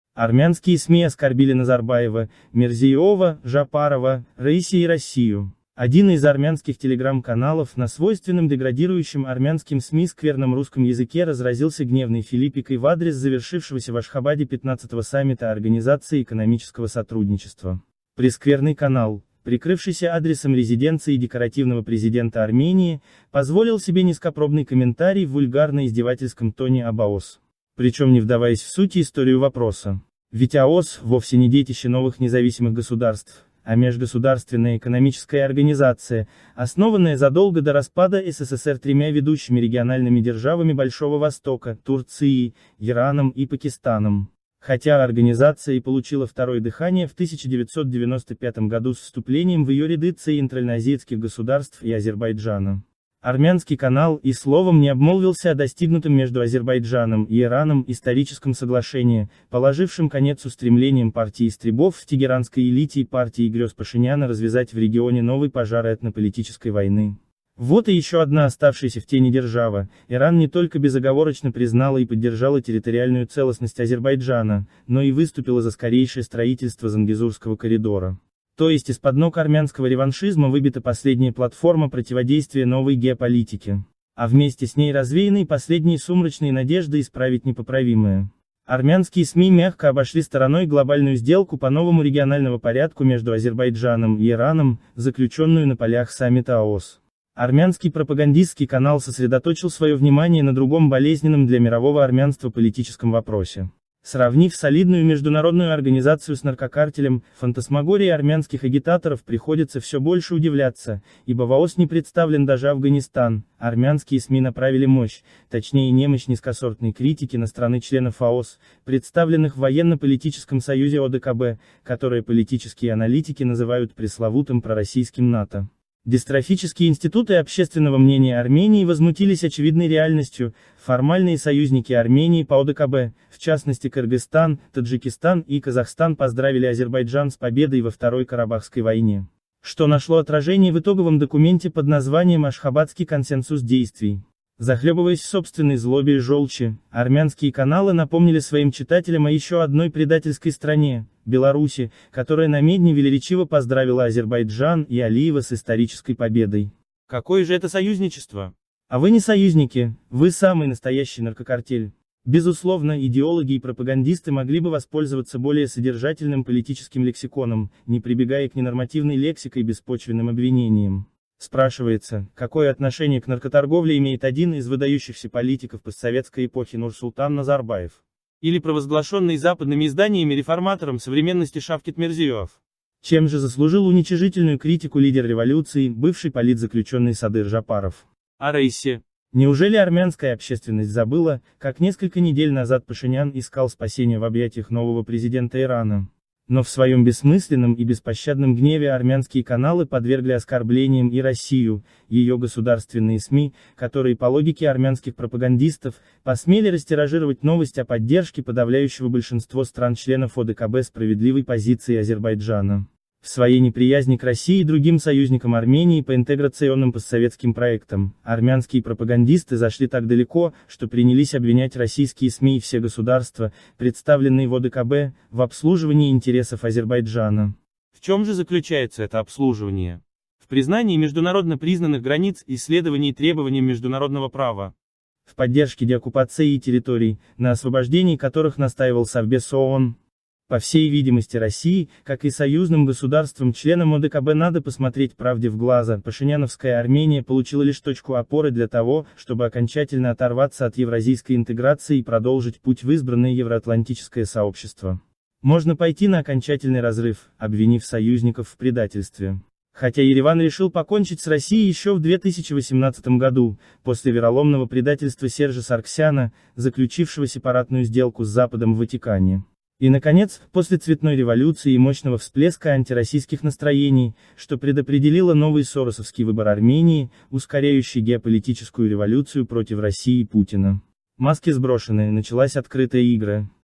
Армянские СМИ оскорбили Назарбаева, Мирзиеёва, Жапарова, Раиси… и Россию
Один из армянских телеграмм-каналов на свойственном деградирующим армянским СМИ скверном русском языке разразился гневной филиппикой в адрес завершившегося в Ашхабаде 15-го саммита Организации экономического сотрудничества. Прескверный канал, прикрывшийся адресом резиденции декоративного президента Армении, позволил себе низкопробный комментарий в вульгарно-издевательском тоне об ОЭС.